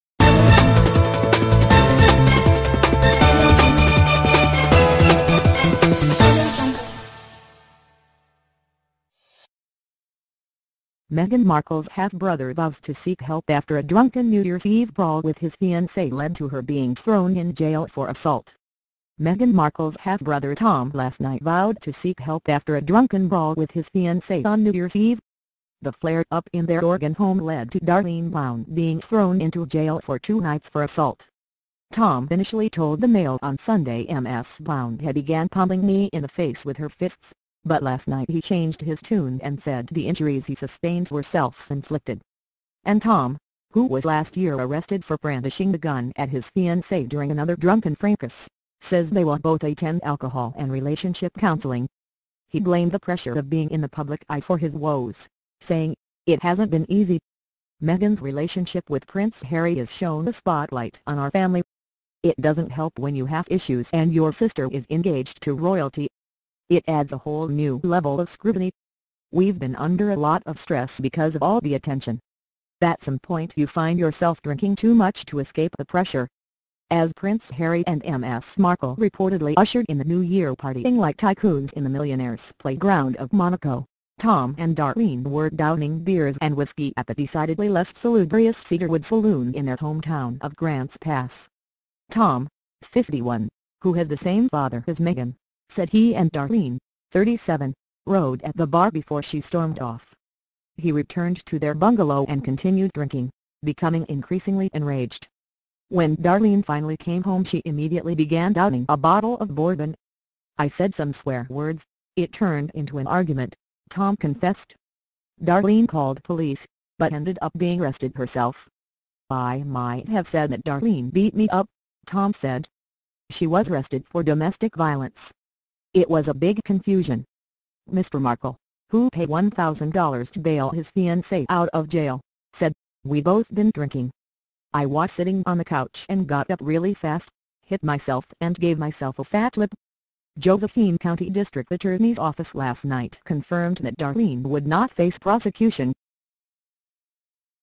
Meghan Markle's half brother vows to 'seek help' after a drunken New Year's Eve with his fiancee
Meghan Markle’s half-brother Tom last night vowed to ‘seek help’ after a drunken  with his fiancee on New Year’s Eve.